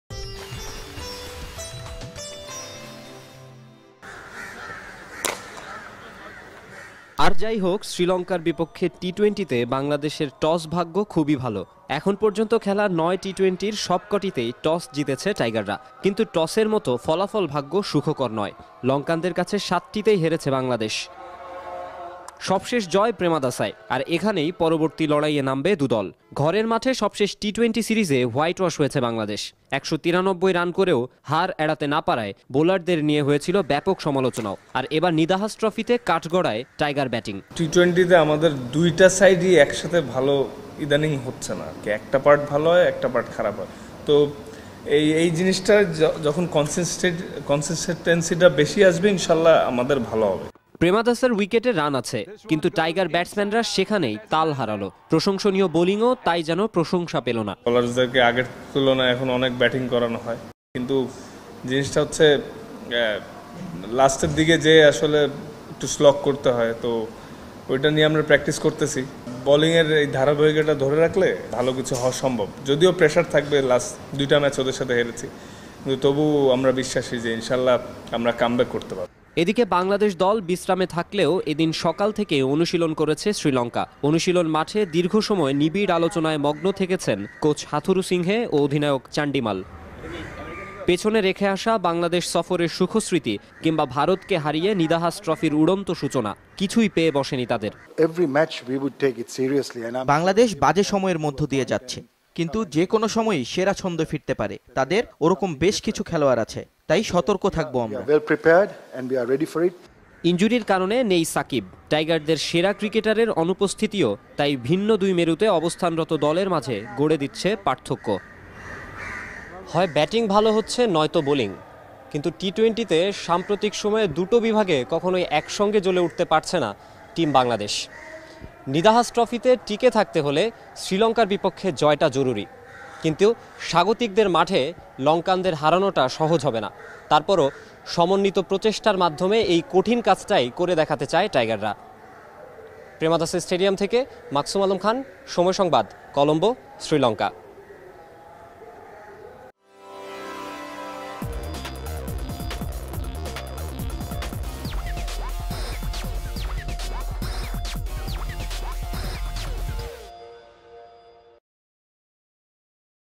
কেন সফলতা পাচ্ছে না বাংলাদেশ, জানালেন তাসকিন | BD Cricket News | Sport News
আর যাই হোক শ্রীলঙ্কার বিপক্ষে টি-টোয়েন্টিতে বাংলাদেশের টস ভাগ্য খুবই ভালো। এখন পর্যন্ত খেলা ৯ টি-টোয়েন্টির সবকটিতেই টস জিতেছে টাইগাররা। কিন্তু টসের মতো ফলাফল ভাগ্য সুখকর নয়। লঙ্কানদের কাছে ৭টিতেই হেরেছে বাংলাদেশ। তবে ২ জয় এসেছে গেলো ২ বছরেই।

সবশেষ জয় প্রেমাদাসায়। আর এখানেই পরবর্তী লড়াইয়ে নামবে দু'দল। ঘরের মাঠে সবশেষ টি-টোয়েন্টি সিরিজে হোয়াইটওয়াশ হয়েছে বাংলাদেশ। ১৯৩ রান করেও হার এড়াতে না পারায় বোলারদের নিয়ে হয়েছিলো ব্যাপক সমালোচনাও। আর এবার নিদাহাস ট্রফিতে কাঠগড়ায় টাইগার ব্যাটিং। ক্রিকেটের সংক্ষিপ্ত ফরম্যাটে যেন ঠিকঠাক মানিয়ে নিতে পারছেনা বাংলাদেশ।

শুক্রবার সংবাদ সম্মেলনে পেসার তাসকিন আহমেদ বলেন, 'দুই টা পার্টই একসঙ্গে ভালো হচ্ছে না। একটা ভালো হচ্ছে তো অন্যটা খারাপ হচ্ছে। এই জিনিসটাতে ধারাবাহিকতা যখন বেশি আসবে তখন ভালো ফল আসবে।'

প্রেমাদাসার উইকেটে রান আছে। কিন্তু, টাইগার ব্যাটসম্যানরা সেখানেই তাল হারালো। প্রশংসনীয় বোলিংও তাই যেন প্রশংসা পেলো না। একটু আফসোস আছে তাসকিনের।

তাসকিন বলেন, 'শেষের দিকে গিয়ে আসলে একটু স্লগ করতে হয়। ওটা নিয়ে আমরা অনুশীলন করছি। তবুও কাজটা অত সহজ না যতটা মনে হয়। তবে আশা করছি, ভবিষ্যতে আরও উন্নতি হবে। আসলে মূল বিষয়টা হলো আমরা যদি শুরুটা ভালো করি তাহলে শেষের দিকেও ব্যাটিংটা ভালো হয়।'

এদিকে, বাংলাদেশ দল বিশ্রামে থাকলেও এদিন সকাল থেকেই অনুশীলন করেছে শ্রীলঙ্কা। অনুশীলন মাঠে দীর্ঘ সময় নিবিড় আলোচনায় মগ্ন থেকেছেন কোচ হাথুরুসিংহে ও অধিনায়ক চান্ডিমাল। পেছনে রেখে আসা বাংলাদেশ সফরের সুখস্মৃতি কিংবা ভারতকে হারিয়ে নিদাহাস ট্রফির উড়ন্ত সূচনা কিছুই পেয়ে বসেনি তাদের। বরং অখন্ড মনোযোগ লঙ্কানদের। প্রতিপক্ষকে সুযোগ দেবেনা একটুও।

লঙ্কানদের পেস বোলিং কোচ রুমেশ রত্নায়েকে বলেন,  'বাংলাদেশ দলের সামর্থ্য আমরা জানি। ওরা বাজে সময়ের মধ্য দিয়ে যাচ্ছে, ঠিক। কিন্তু, যেকোনো সময়ই সেরা ছন্দে ফিরতে পারে। তাই সতর্ক থাকবো আমরা।'

 ইনজুরির কারণে সাকিব নেই। টাইগারদের সেরা ক্রিকেটারের অনুপস্থিতিও তাই ভিন্ন দুই মেরুতে অবস্থানরত দলের মাঝে গড়ে দিচ্ছে পার্থক্য।